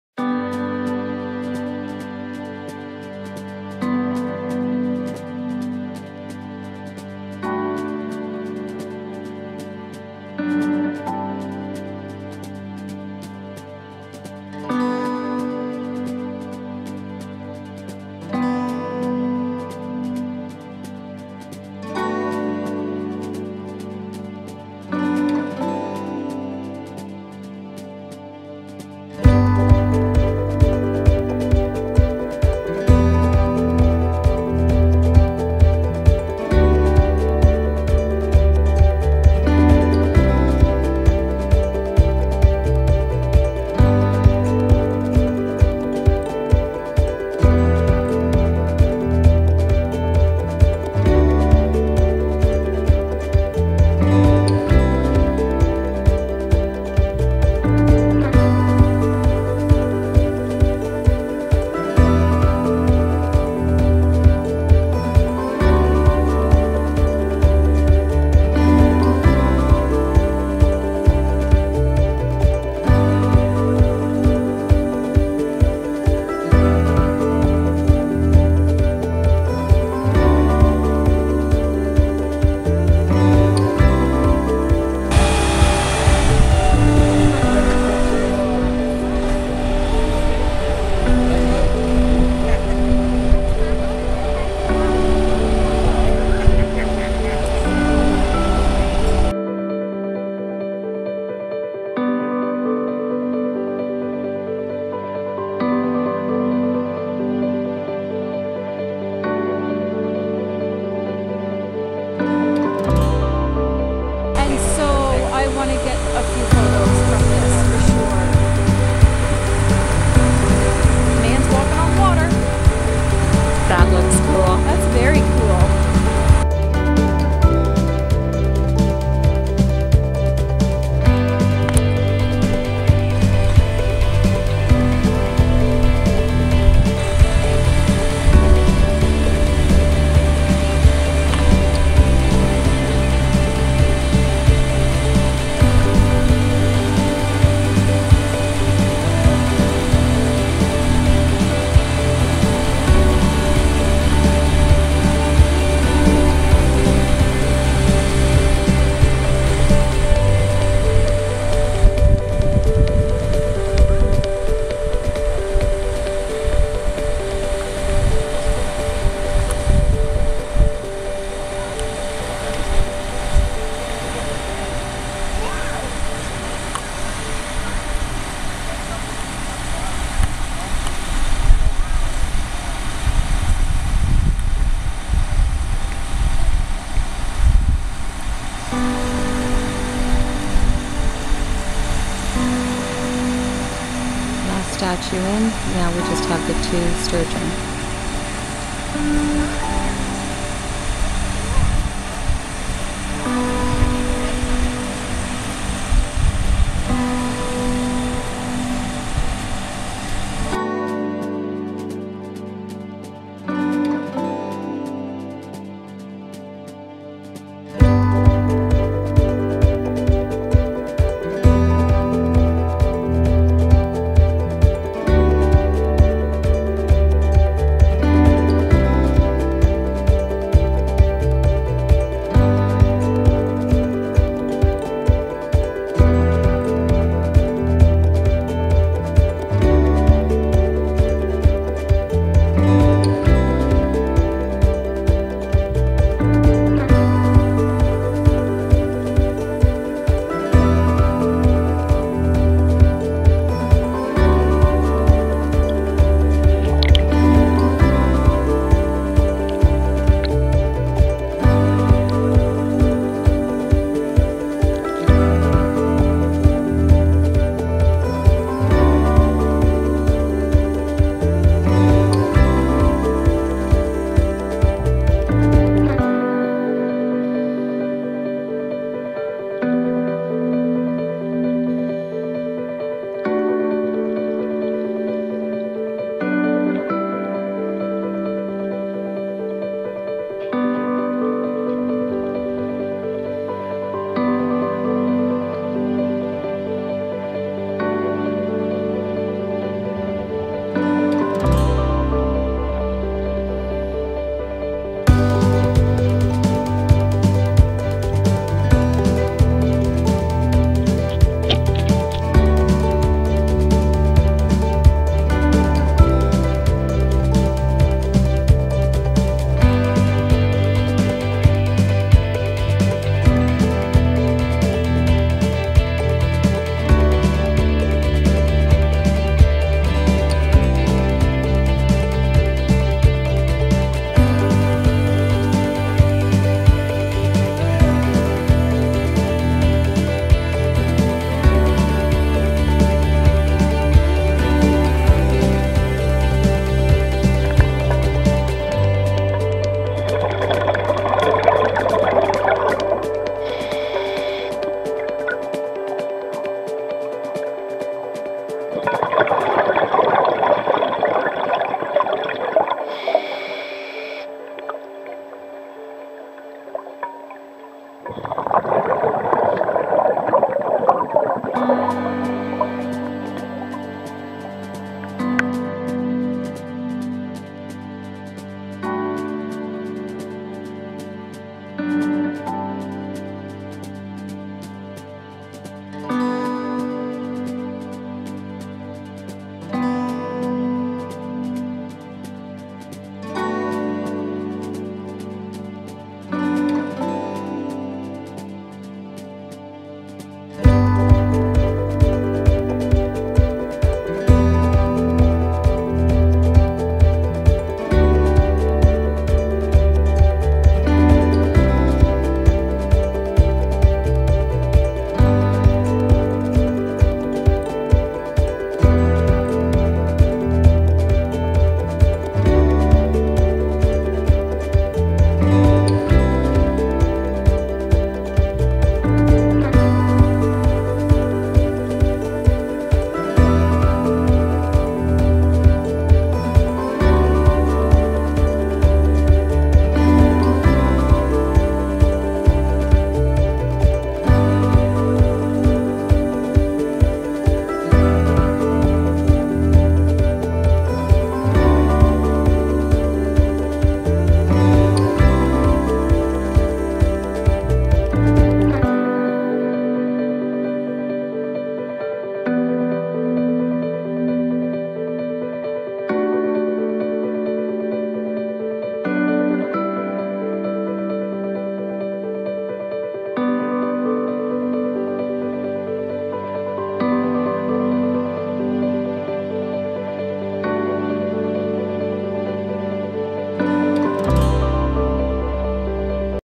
Centeen Memorial Park Previous Statue Installs
Brief video of statue installations over the past 5 years by SOS Thousand Islands.